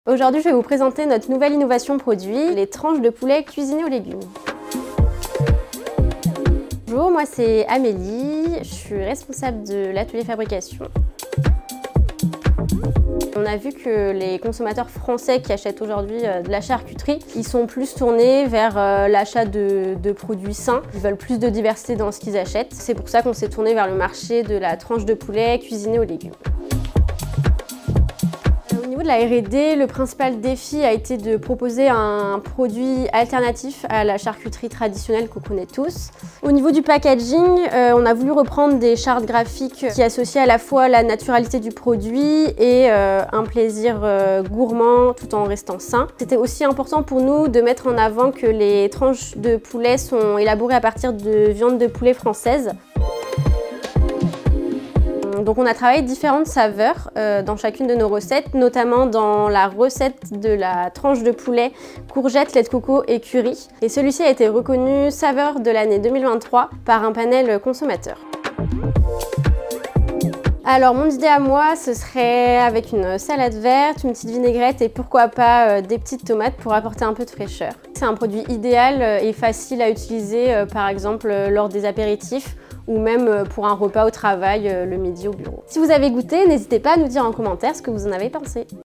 Tranches de poulet cuisiné aux légumes présentées par Amélie, responsable d'atelier chez Guyader
Aujourd’hui, Amélie, responsable de l’atelier de fabrication de Saint Agathon vous présente notre nouvelle innovation produit de cette année 2023, la tranche de poulet aux légumes !
L’innovation est au cœur des valeurs Guyader et nous souhaitons vous partager l’histoire de ce nouveau produit de sa réflexion à sa conception !